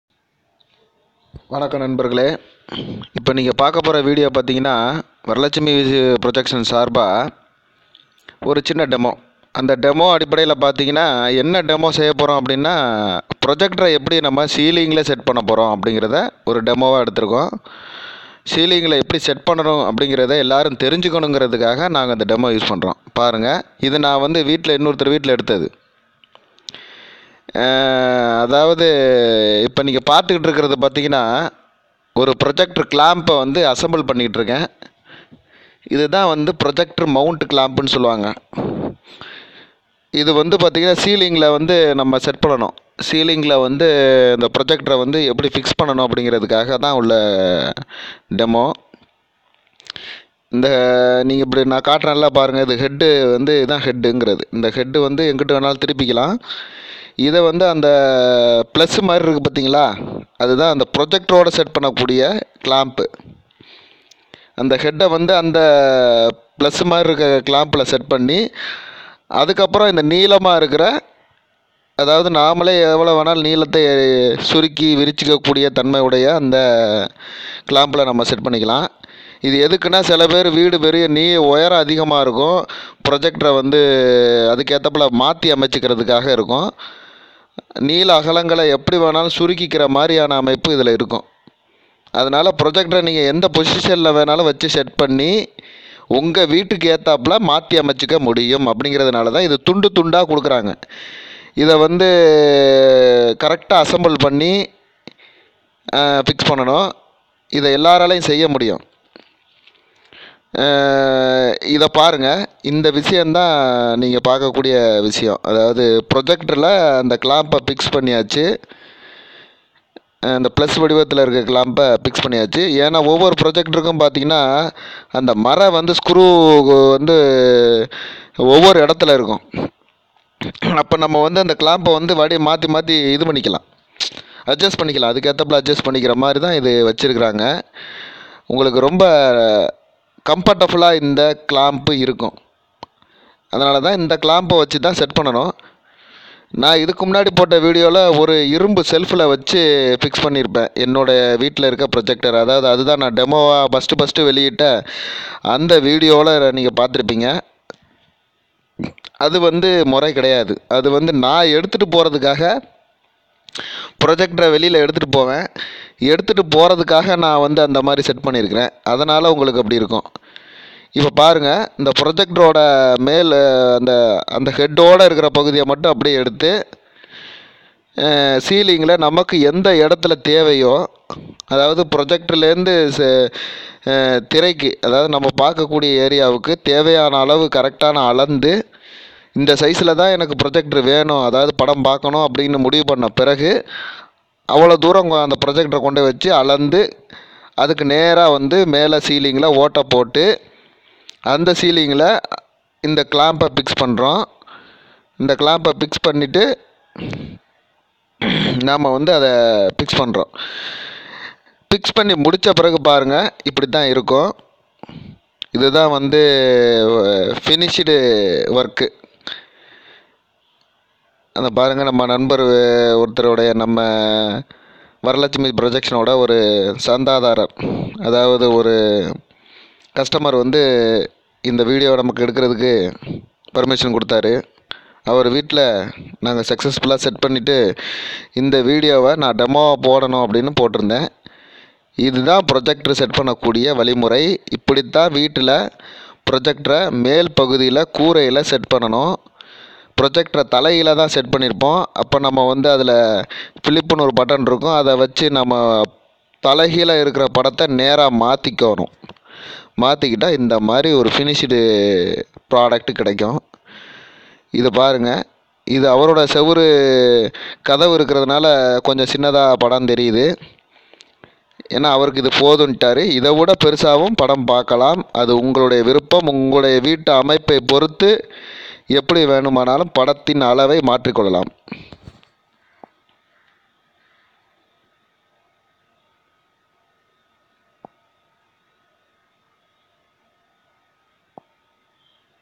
வரலெட்சுமி வீட்டு சினிமா (Projector fixing in seeling)
நண்பர்களே இல்லத்தரசிகளே இந்த காணோளி புரஜக்டரை எப்படி மேற்கூறையில் பொருத்துவது என்பது சம்பந்தமானது இது உங்களுக்கு
பயனுள்ளதாக இருக்கும் என நம்புகிறோம்
மேலும் எங்களது புது காணொளிகள் (வீடியோக்கள்) நாங்கள் போடும் போது
வீடியோ இணைப்பை பகிரவும் உங்கள் ஆதரவுக்கு நன்றிகள் கோடி